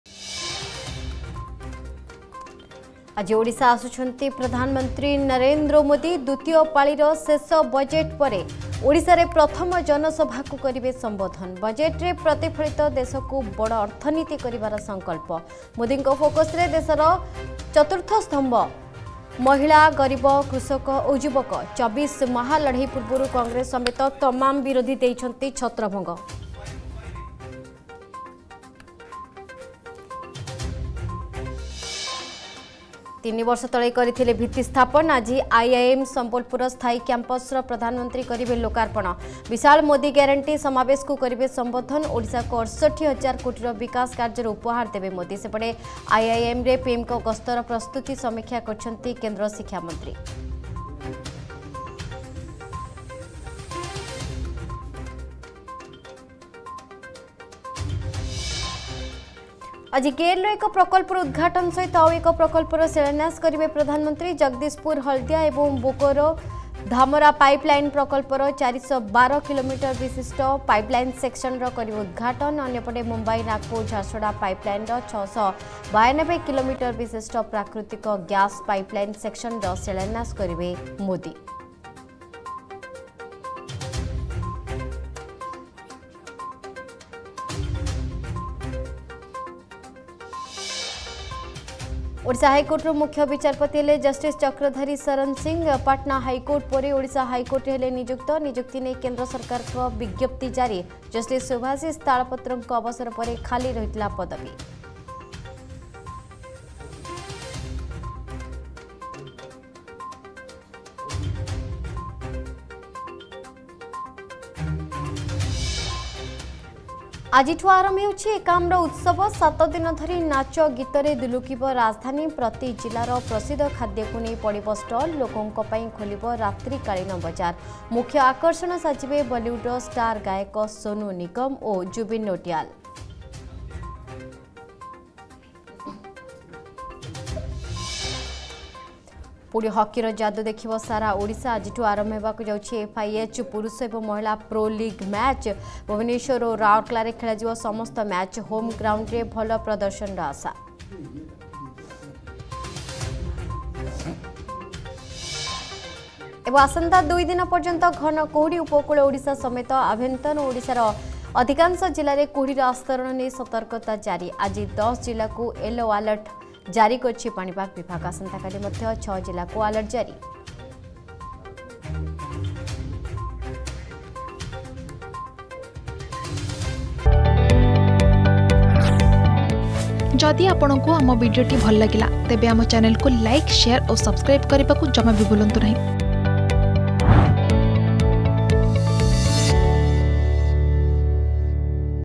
Headlines @7AM | 3 Feb 2024 | Argus News
+ଆଜି ଓଡ଼ିଶା ଆସୁଛନ୍ତି ପ୍ରଧାନମନ୍ତ୍ରୀ ନରେନ୍ଦ୍ର ମୋଦି
+ଆଜି ଗେଲ୍ ର ଏକ ପ୍ରକଳ୍ପର ଉଦଘାଟନ ସହ ଆଉ ଏକ ପ୍ରକଳ୍ପର ଶିଳାନ୍ୟାସ କରିବେ ପ୍ରଧାନମନ୍ତ୍ରୀ